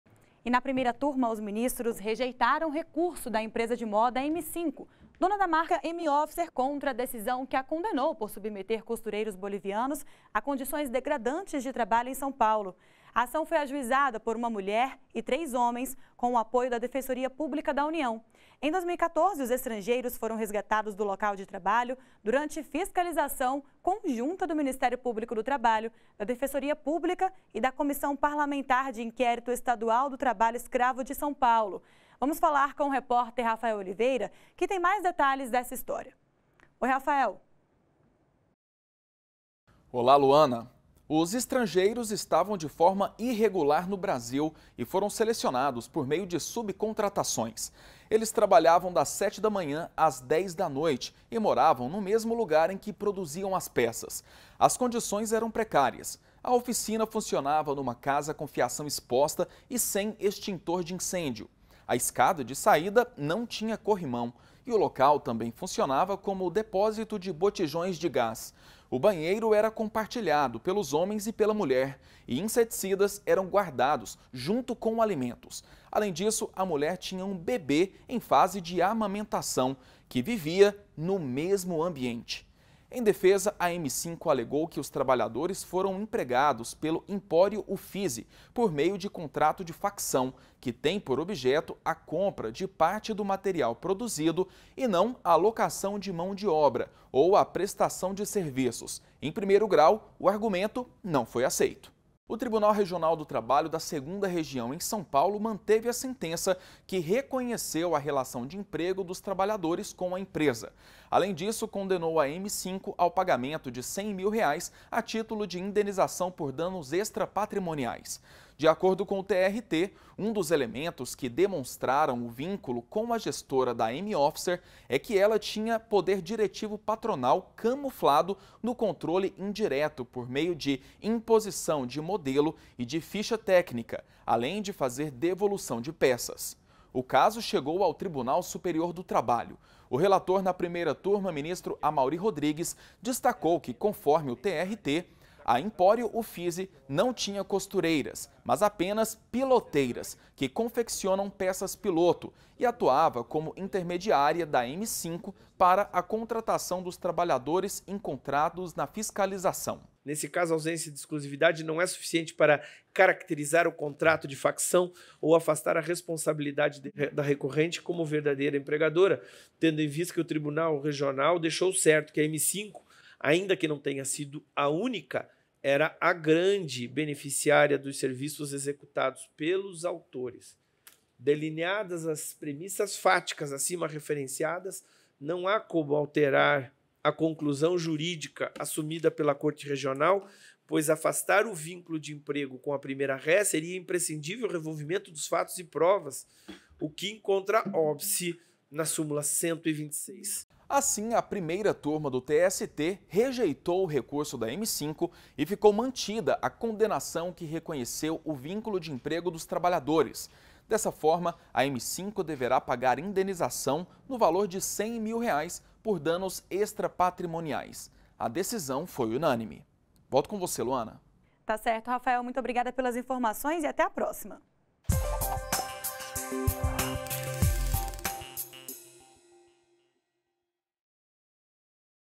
Empresa de moda é condenada por submeter trabalhadores bolivianos a condições degradantes
A Primeira Turma do Tribunal Superior do Trabalho rejeitou recurso da M5 Indústria e Comércio Ltda. (M. Officer) contra decisão que a condenou por manter quatro costureiros bolivianos trabalhando em condições degradantes em São Paulo (SP).

Os juízos de primeiro e segundo graus reconheceram a relação de emprego e determinaram o pagamento de indenizações por danos extrapatrimoniais. No TST, o colegiado entendeu que, para se chegar a conclusão diversa, seria necessário reanalisar fatos e provas, conduta vedada em recurso de revista.